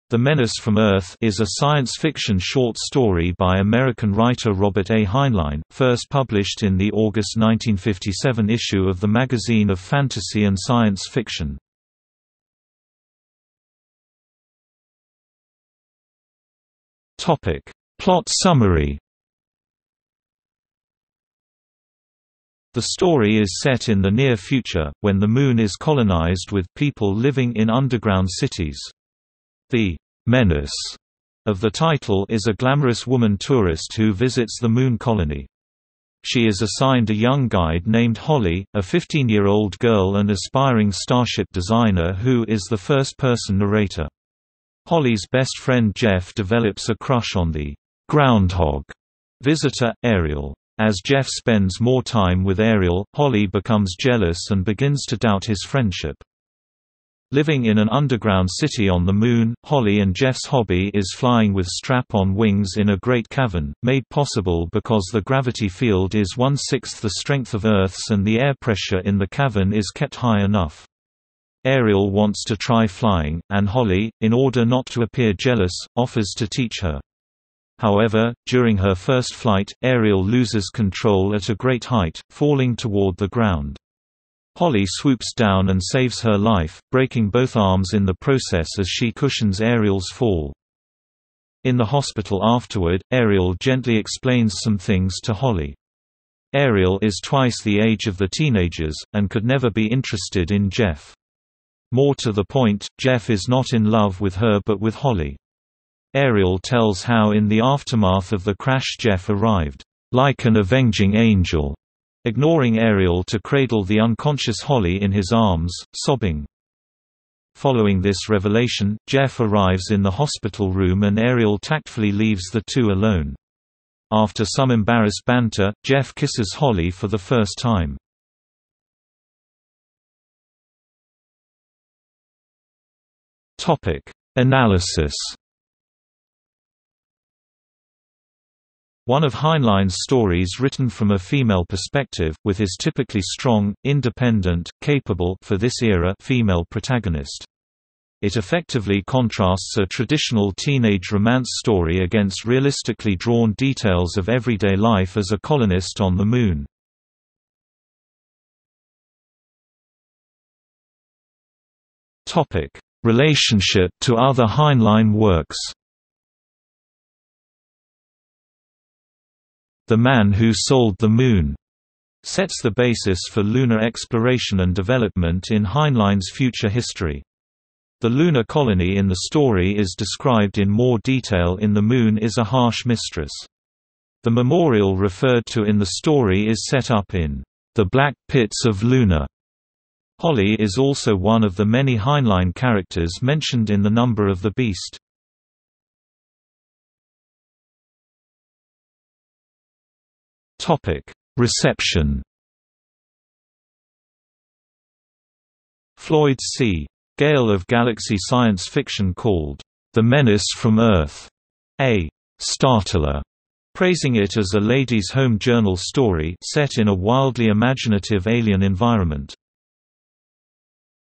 The Menace from Earth | Wikipedia audio article
00:00:13 1 Plot summary
00:02:09 2 Analysis
00:02:38 3 Relationship to other Heinlein works
00:03:16 4 Reception

- Socrates

SUMMARY
"The Menace From Earth" is a science fiction short story by American writer Robert A. Heinlein, first published in the August  1957 issue of The Magazine of Fantasy & Science Fiction.